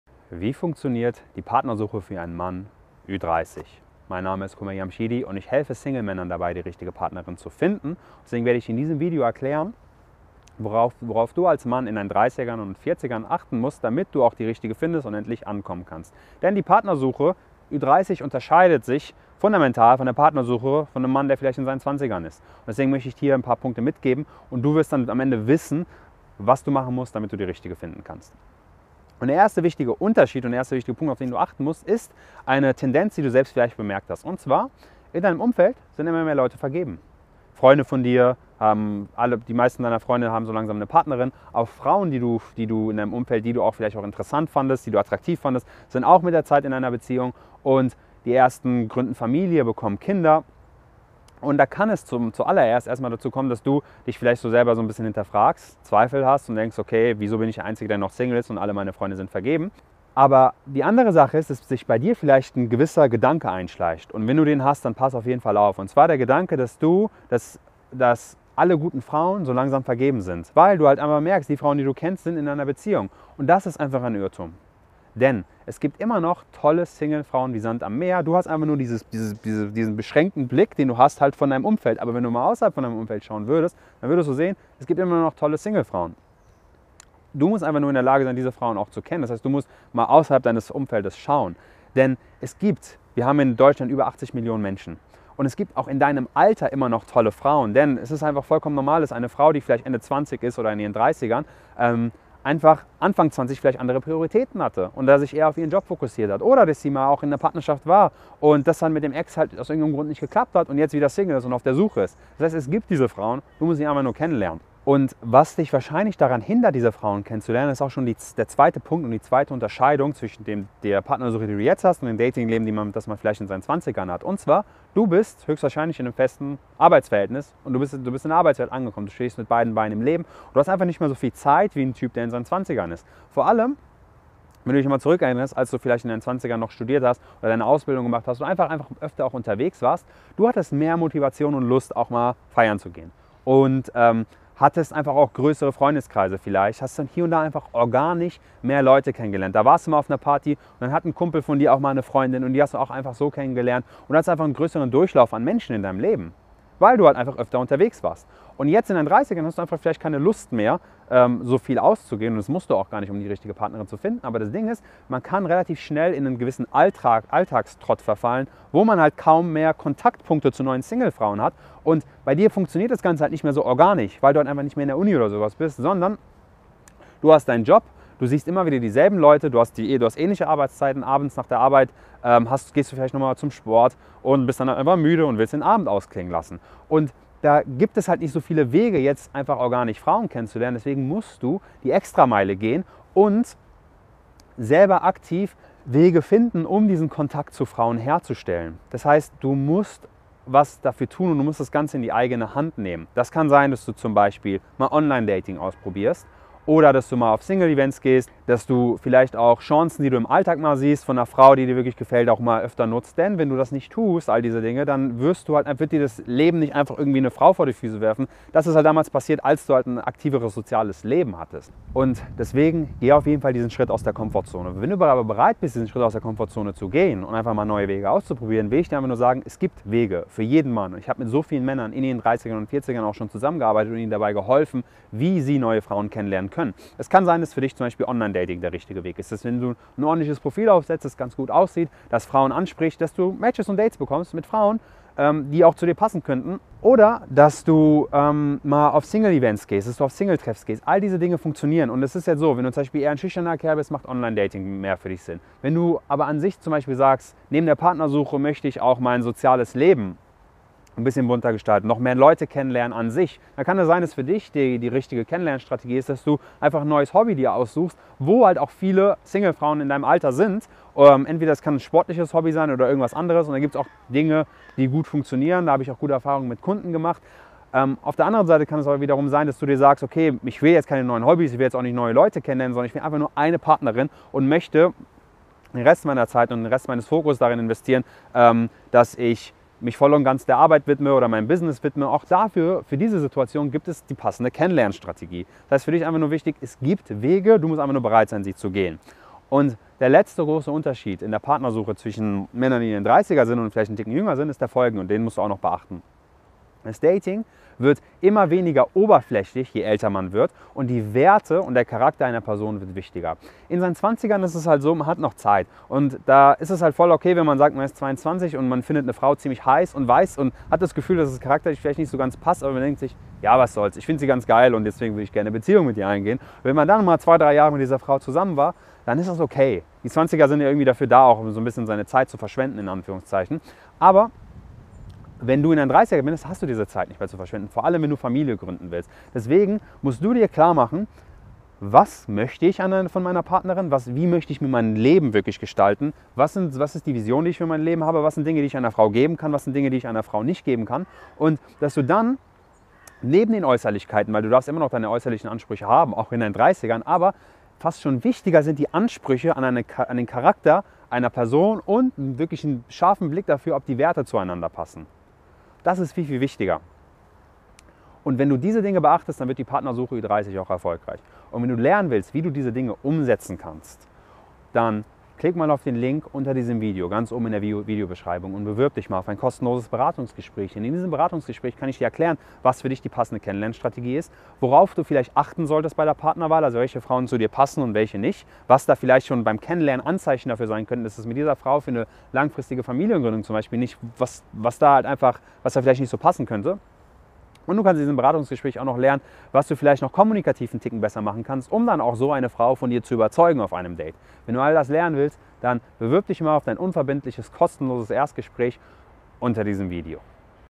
Wie funktioniert die Partnersuche für Männer Ü30?
In diesem Video erkläre ich dir, wie du als Single-Mann, der in seinen 30ern oder 40ern ist, die richtige Partnerin finden kannst. Du wirst lernen, worauf es ankommt, damit du Erfolg bei der Partnersuche hast.